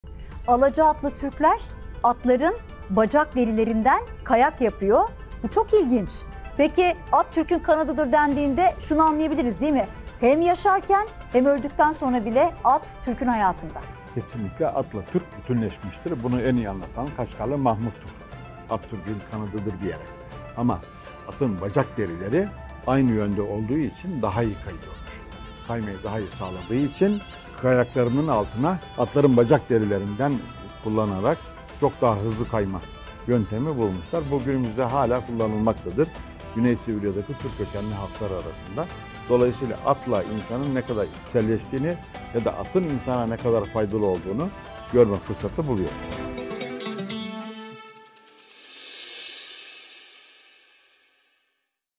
📣 4. Dünya Göçebe Oyunları’nda Pelin Çift ile Soru-Cevap’ın 14. bölümü yayında!
💬 Pelin Çift sordu: "Eski Türklerde Atların Önemi Nedir?"

🎙 Prof. Dr. Ahmet Taşağıl cevapladı.